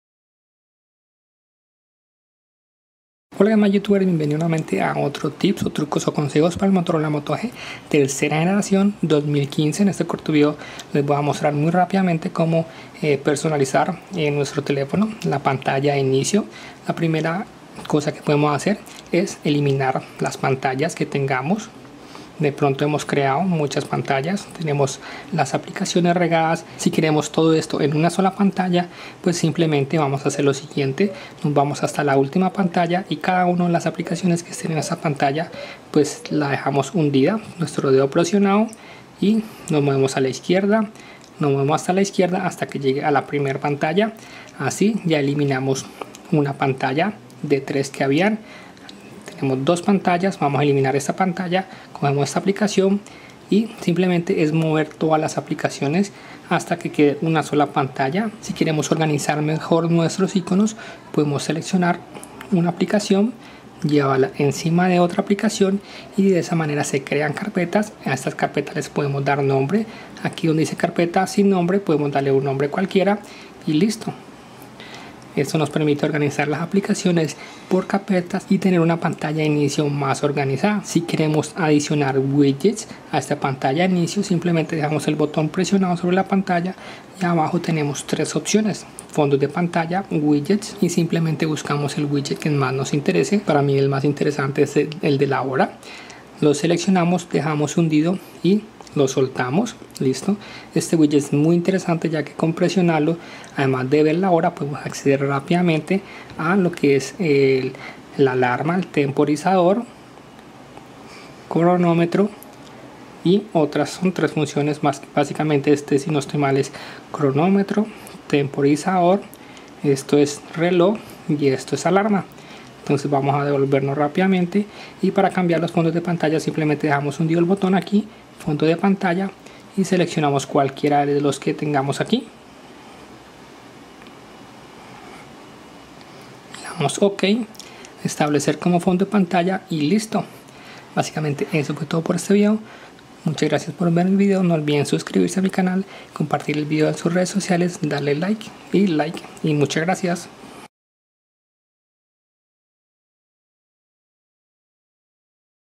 Moto G 2015 Trucos y Tips Parte 13 Personalizar Android
Cómo personalizar y mejorar el aspecto de Android Consejos Utiles Moto G 2015 Tercera Generacion Trucos y funciones ocultas para sacar más provecho de tu Moto G 2015 Parte 13 Suscríbete al Canal